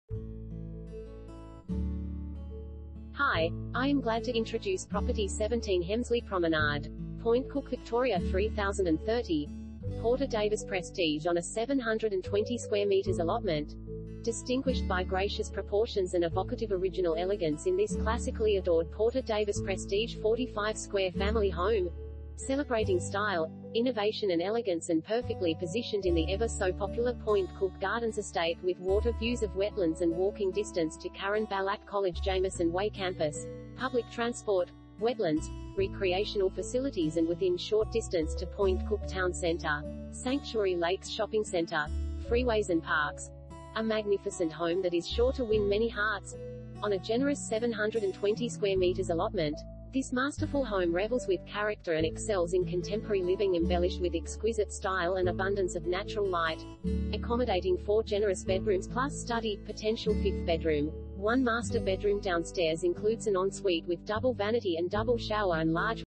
17 Hemsley Promenade, Point Cook VIC 3030 | AuPropertyReport.Com
17 Hemsley Promenade, Point Cook VIC 3030
Property Type: house
Bedrooms: 4
Bathrooms: 4
Car Space: 2
Porter Davis Prestige on a 720m2 allotment!